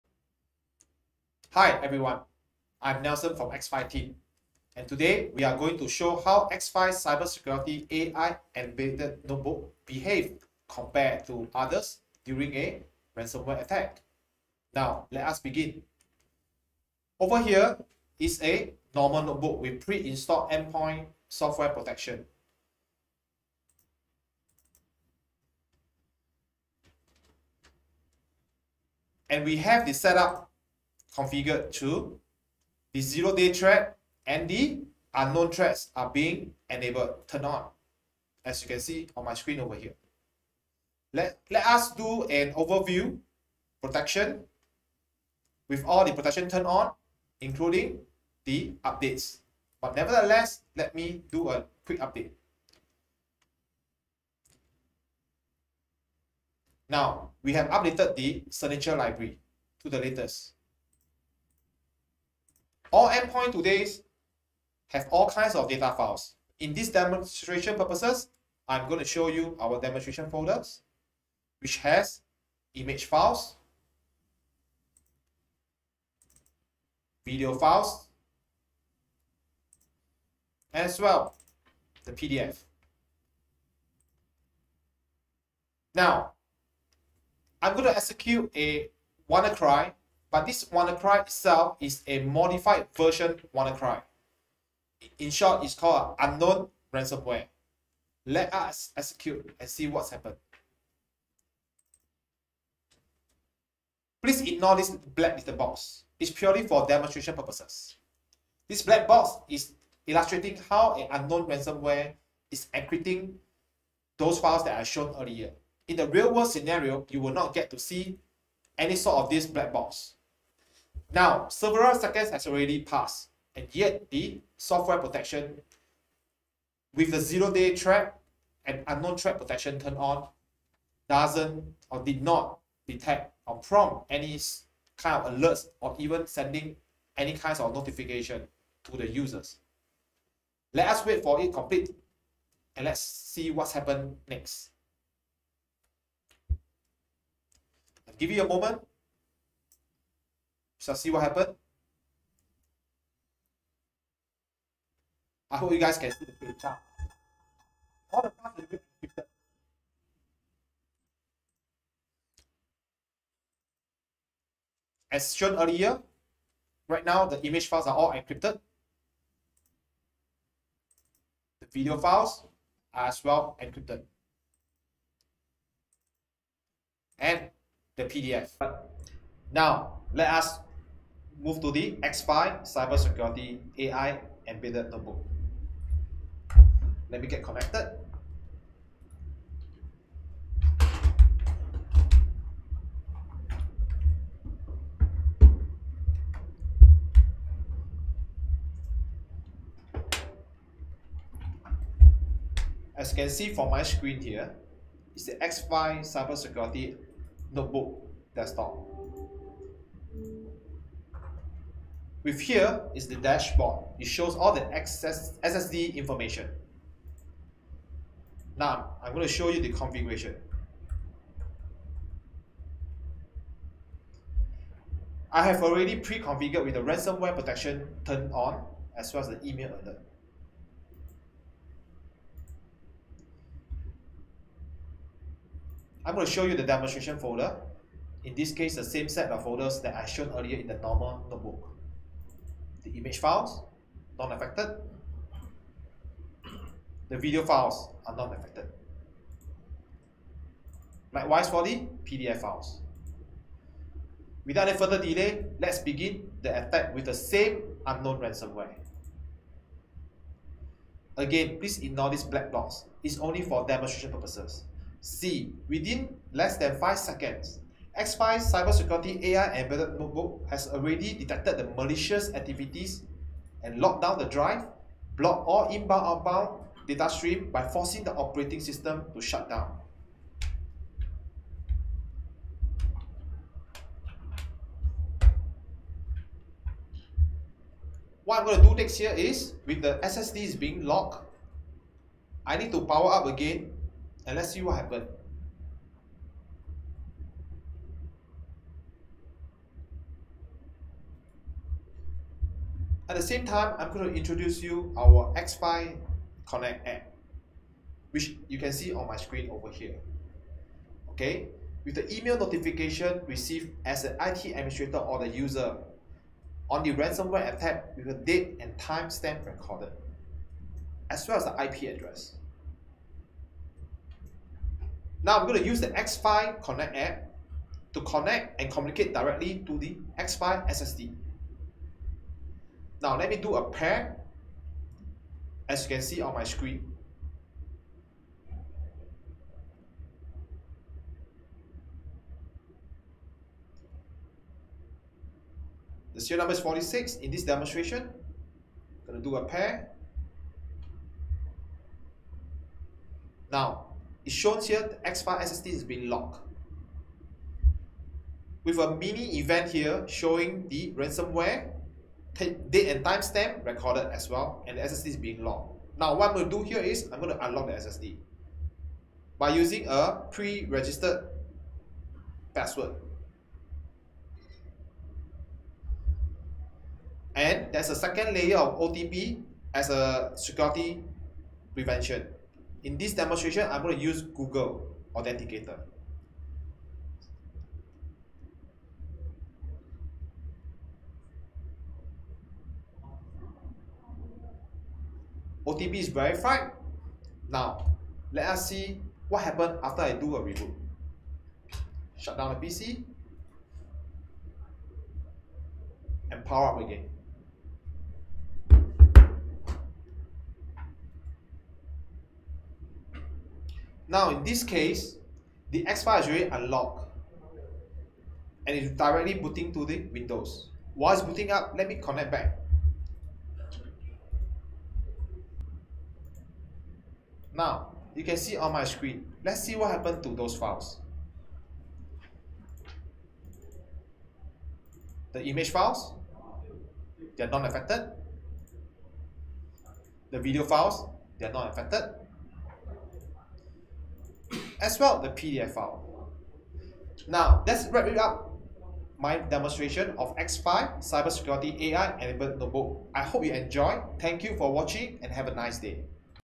X PHY® Ransomware Attack Demonstration
A short demonstration on how X-PHY® reacts to a Ransomware Attack